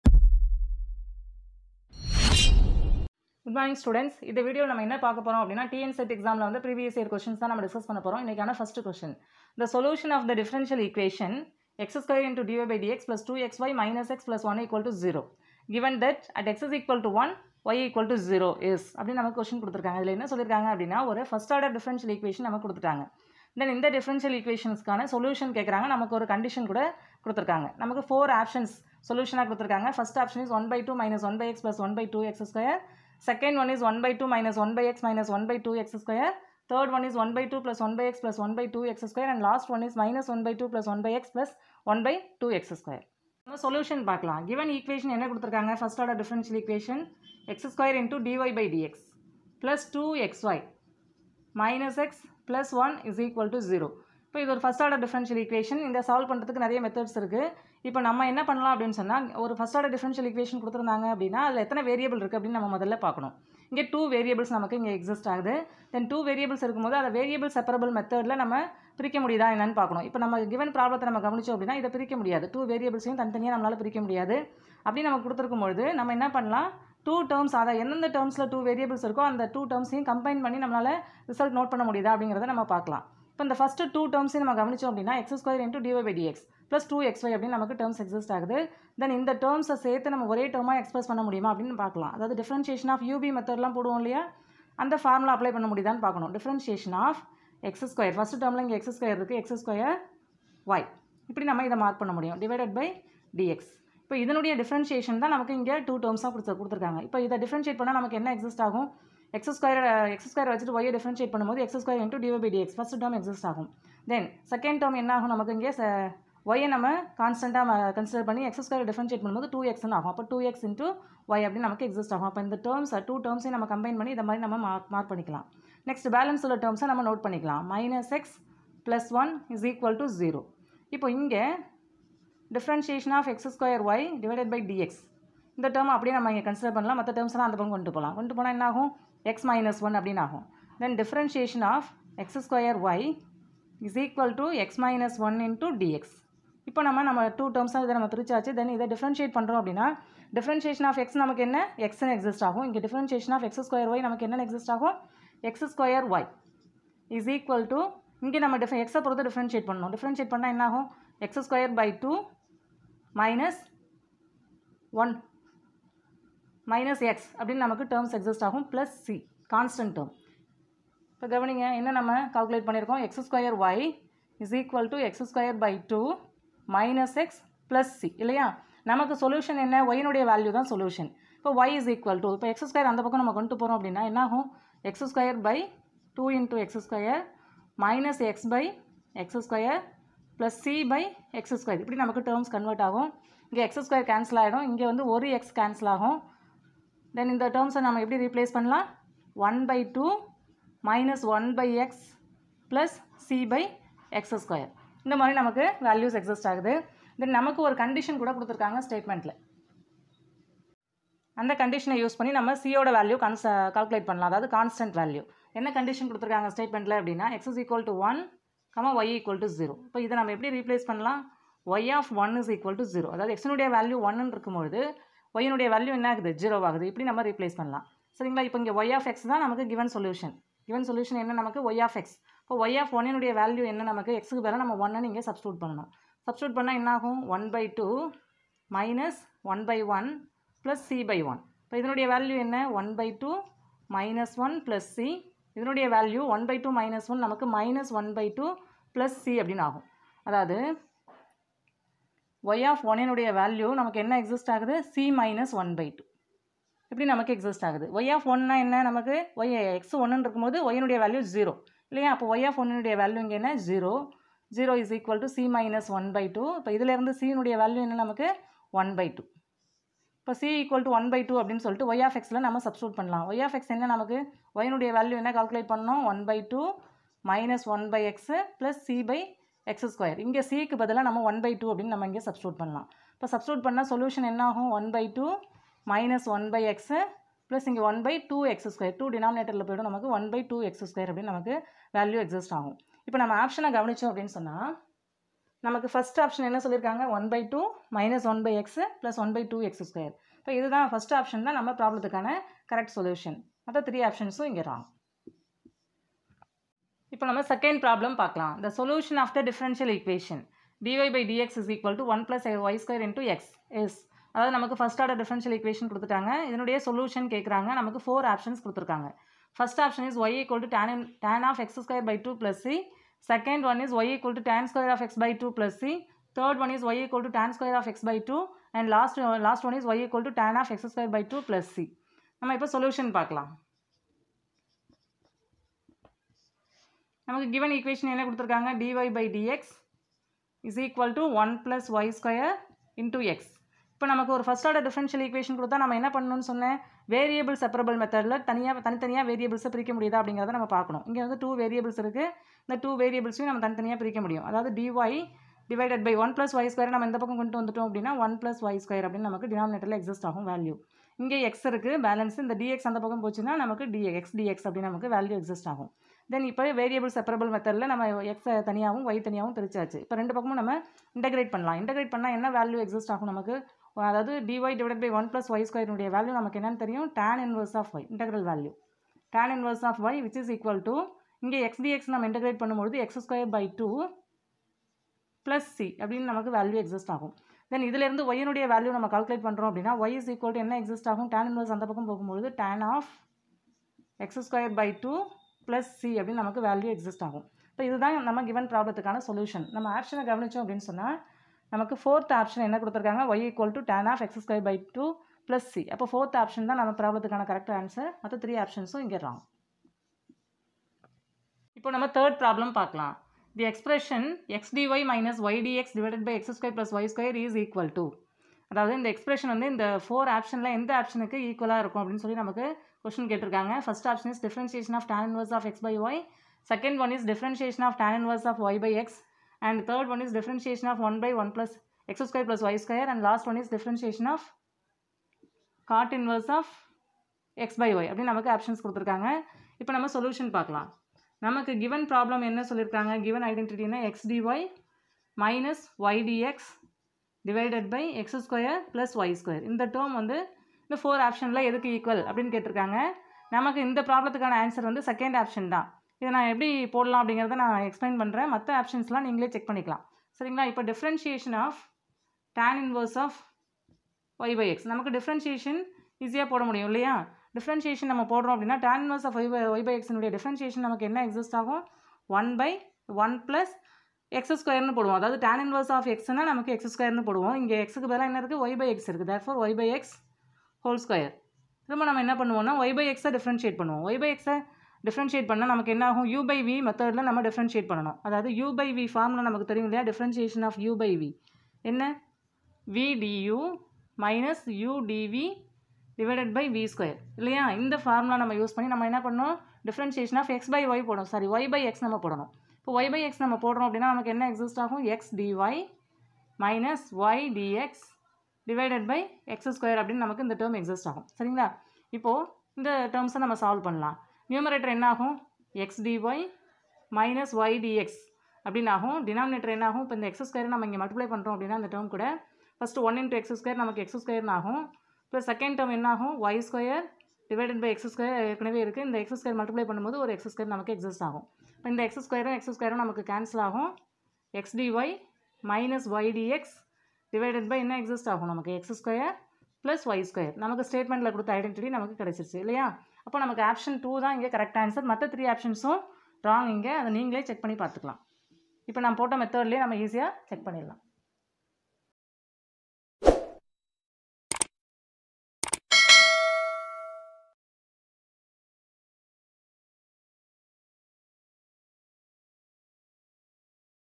TNSET MATHEMATICS/FIRST ORDER DIFFERENTIAL EQUATIONS
In this video we have discussed tnset previous year questions, solution for first order differential equations